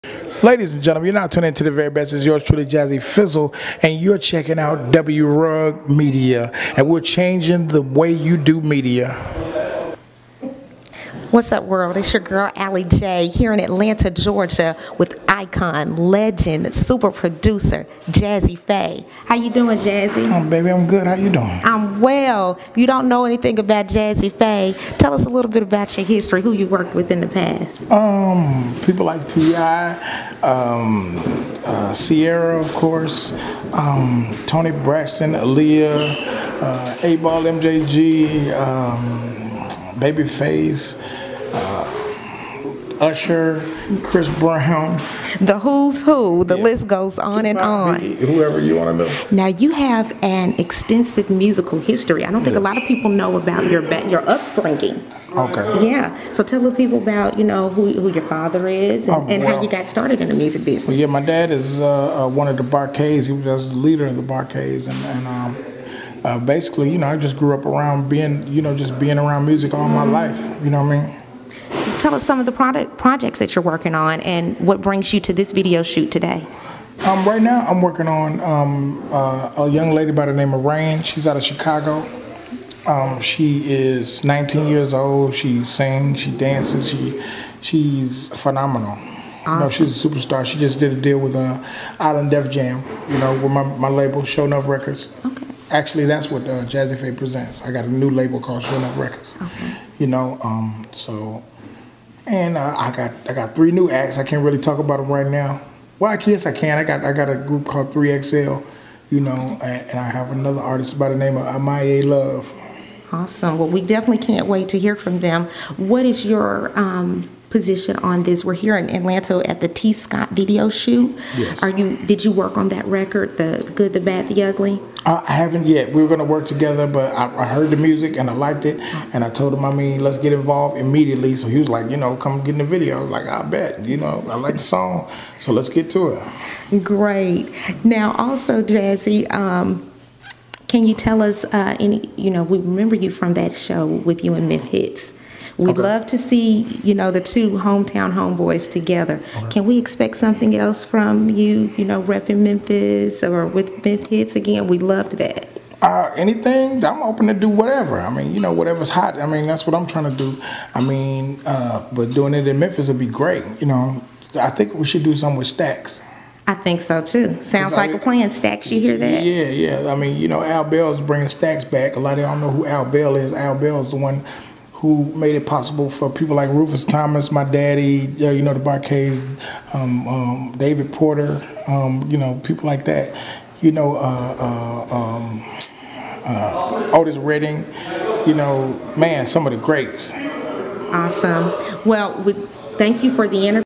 Jazze Pha Talks About Memphitz, TI and Possible Reality Show
We caught up with Jazze Pha at the T Scott Video in Atlanta Georgia. He talks about who he's worked with in the industry, his new artists and his interest having his own Memphis Based reality show